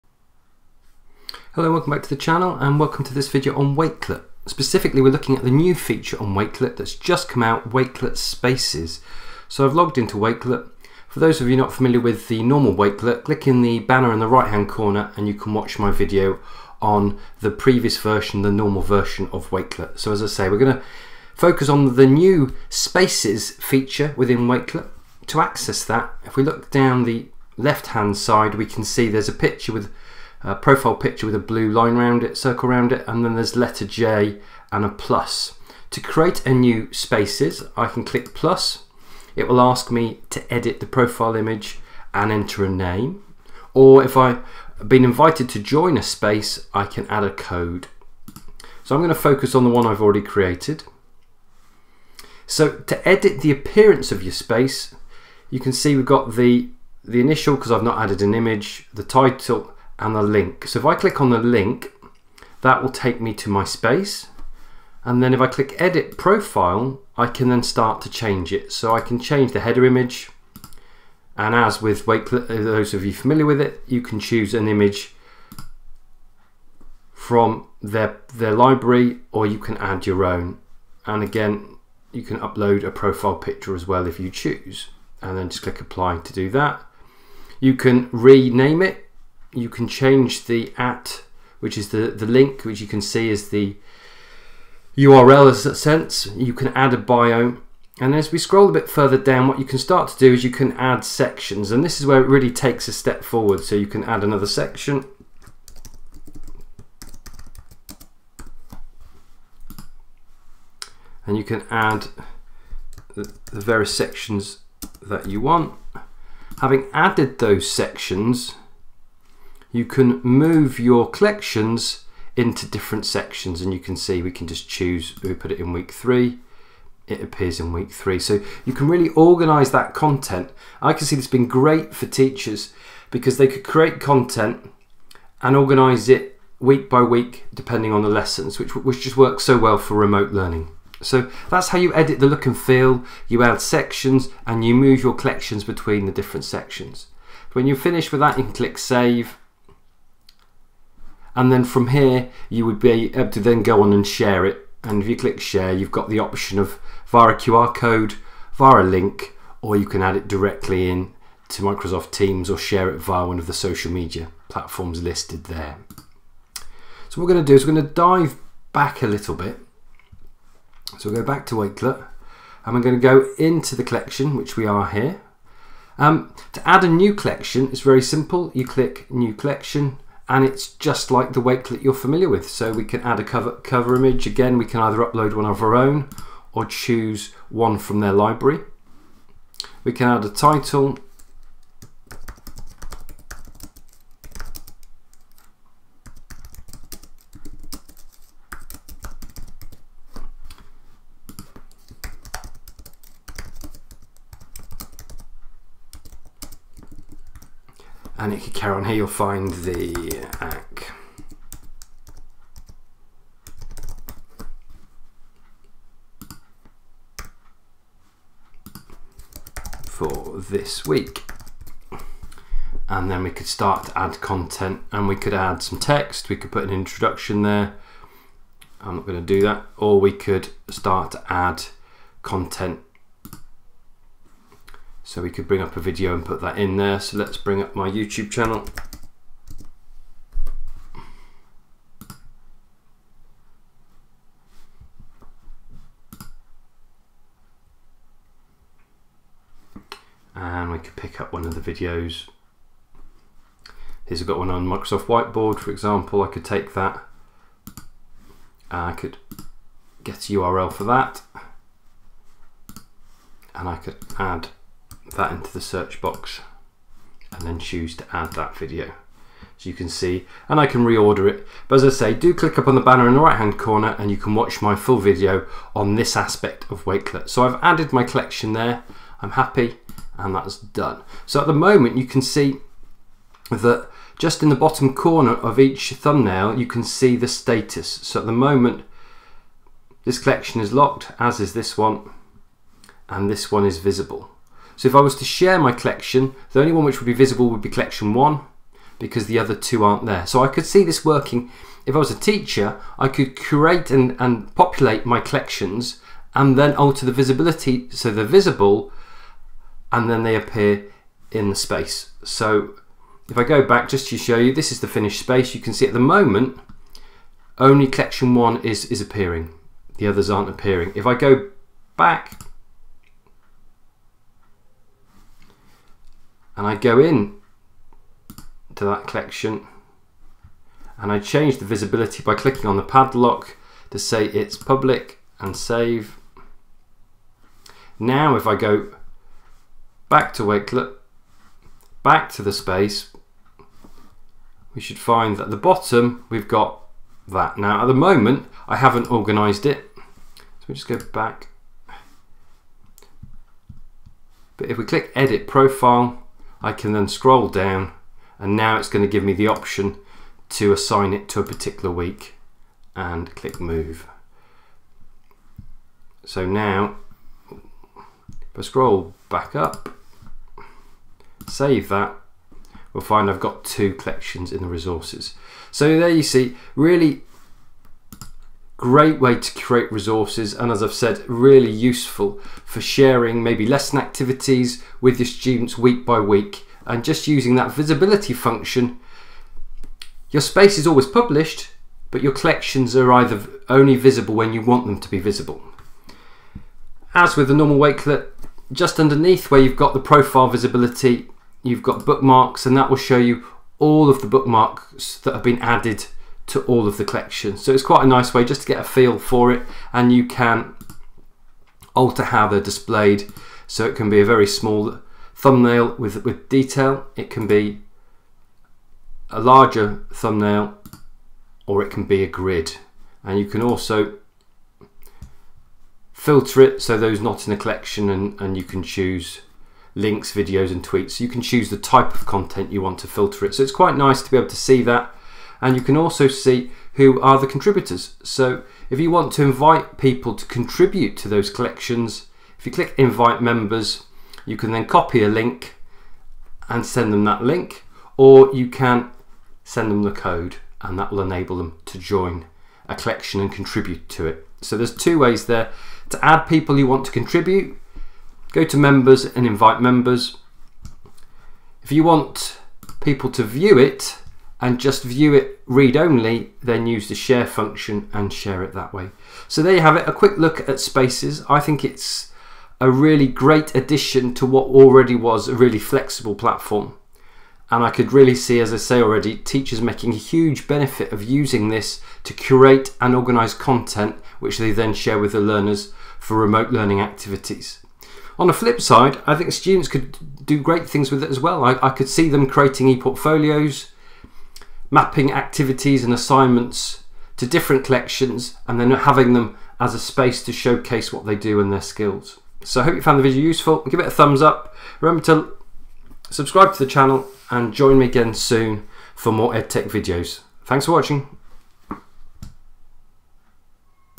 Wakelet Spaces a user guide for teachers
In this video I go through how to use the latest addition to Wakelet, Wakelet Space.

Wakelet spaces  takes all the features of Wakelet collections that you're familiar with and enables you to pull together multiple collections into the one space whilst organising them under headings.

 I will highlight it features and share how I think it could used by both teachers and students.